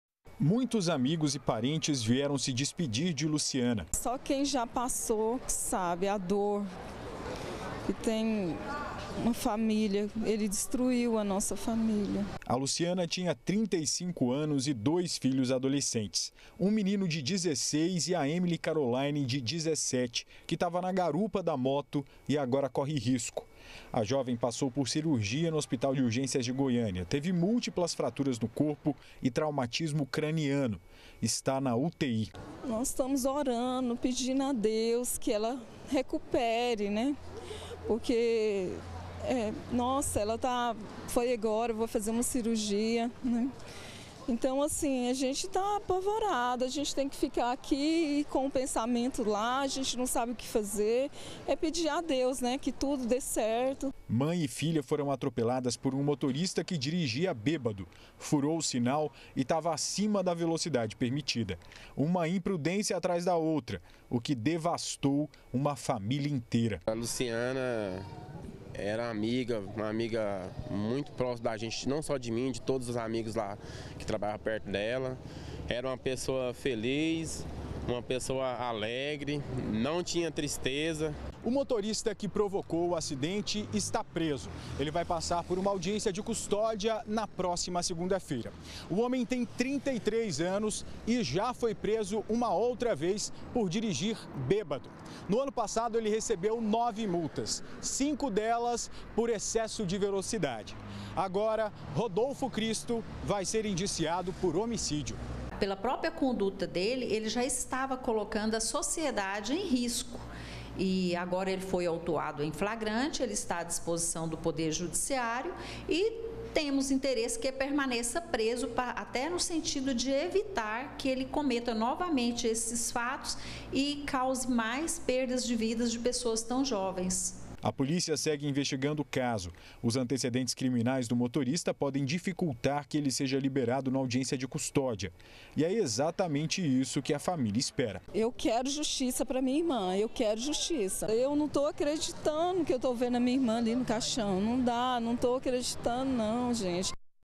GA - Dor no velório de vítima de bêbado - 12-10-2017
O corpo da mulher morta por um motorista bêbado em Aparecida de Goiânia foi velado nessa madrugada. Parentes e amigos de Luciana Sousa se despediram em clima de dor e revolta. A filha dela, que estava na garupa da moto continua internada em estado grave. O motorista foi indiciado por homicídio.